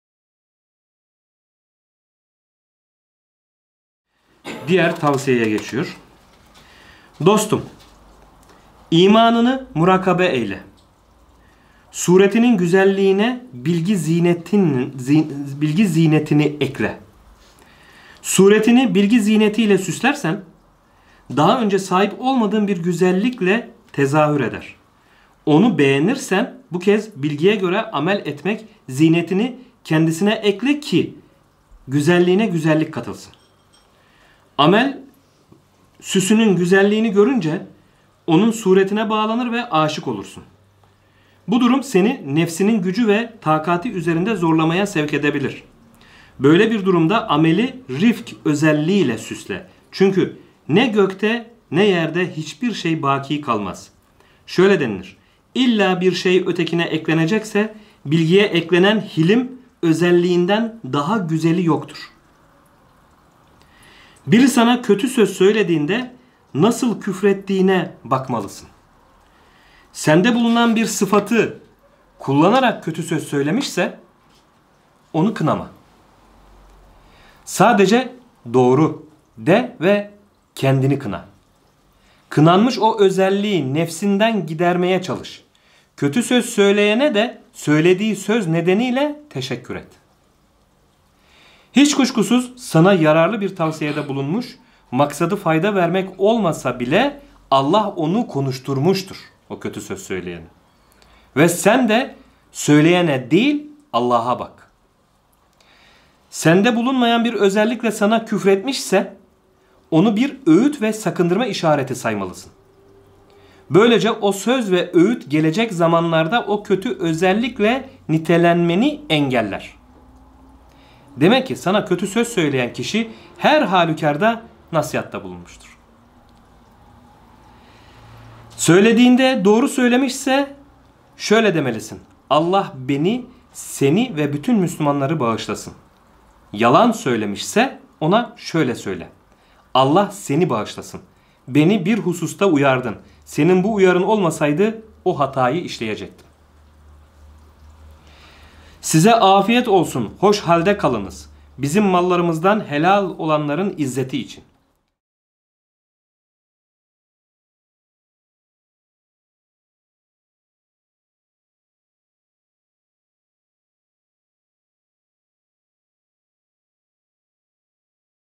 Biri sana kötü söz söylerse, ne yapmalısın?
Videonun Bulunduğu Aralık: Fütûhât-ı Mekkiyye 415. Bölüm Sorular ve Cevapları: 19:17-21:48

Not: Muhyiddin İbnü'l-Arabî Hazretleri (K.S)  Ekolü Üzere
Tasavvuf Sohbetidir.

Facebook: The School of Ibn Arabi

Arapça Yayın Yaptığımız Hesaplar